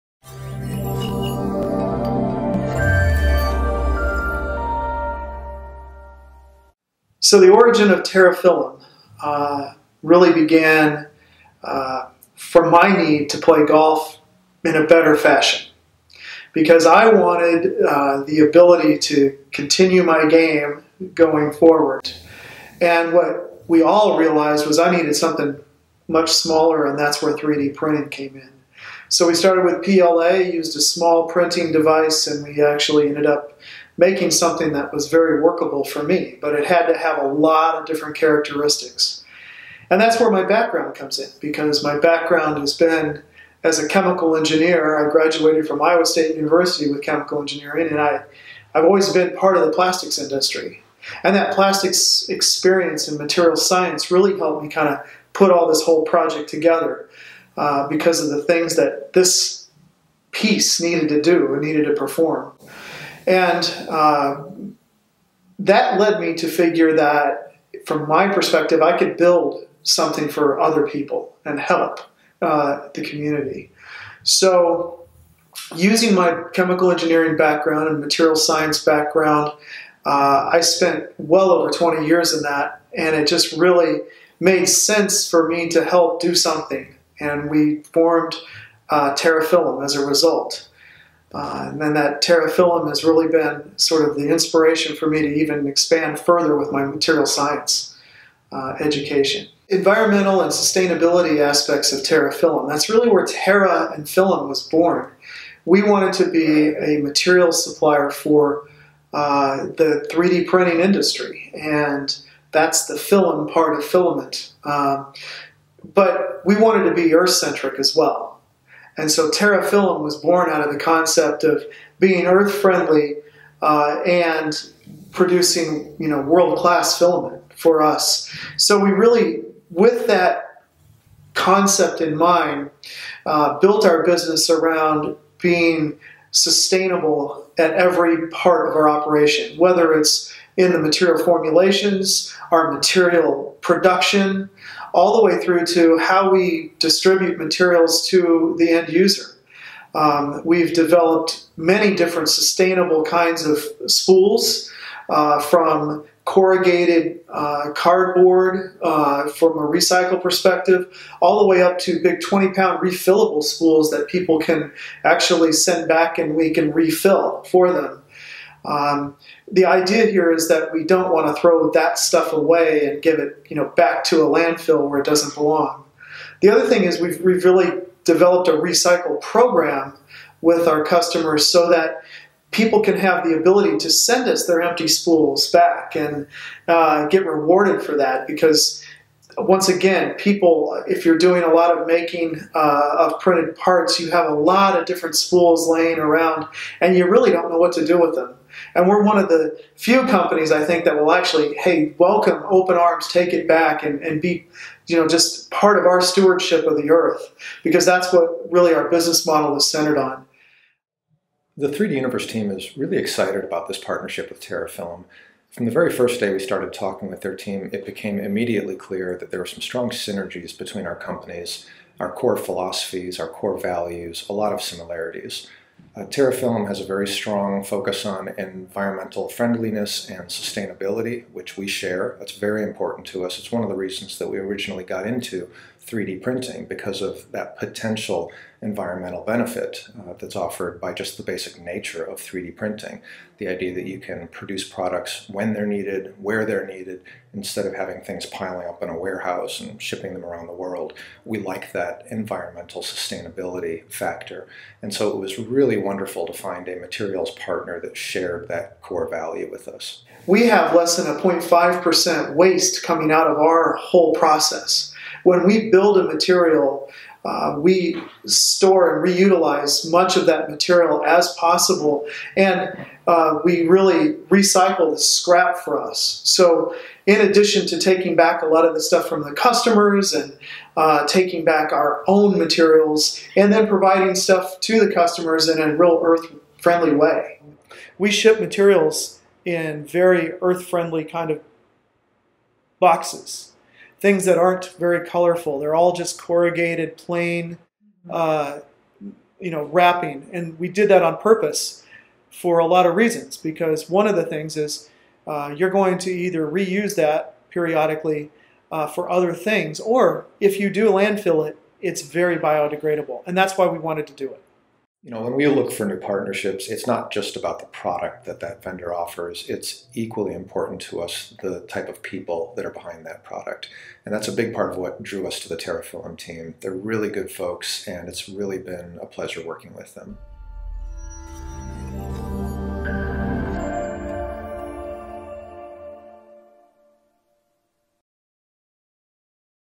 3D Universe Terrafilum 3D Printing Materials Introduction
3D Universe is very excited to introduce our partnership with Terrafilum Engineered Filaments! Terrafilum was started with one idea in mind: to develop and provide 3D printer users the highest-quality, eco-friendly 3D printing filament solutions on the market. We set out to exceed all our customers’ 3D printing needs.

To learn more, or to purchase 3D Universe Terrafilum materials, please